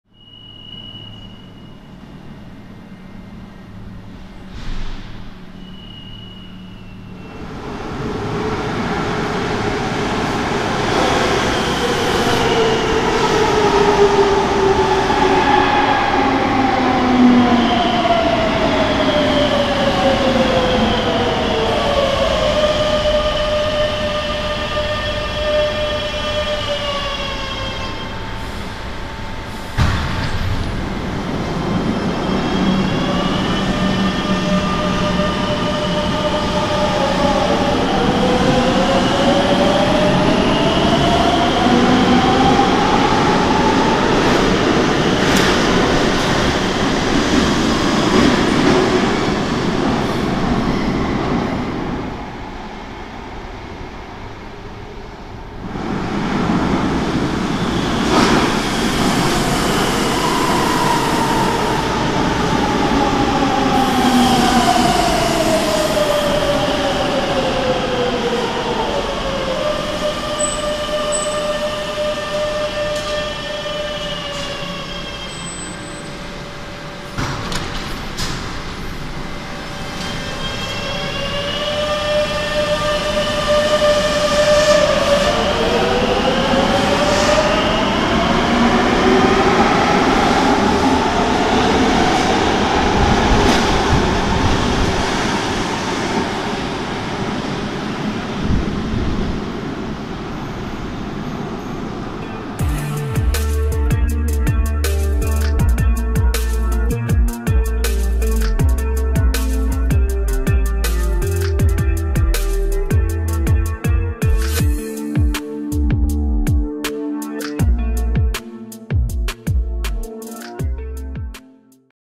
Pueblo Nuevo L7 : Metro de Madrid ( Serie 9000 )
Estación de Pueblo Nuevo de la línea 7 del metro de Madrid. Trenes serie 9000.

- Estación inaugurada en Julio de 1974.
- Correspondencia con la línea 5 de metro.
- La estación se encuentra entre las estaciones de Barrio de la Concepción y Ascao, en el distrito de Ciudad Lineal.

ACC84 (2021)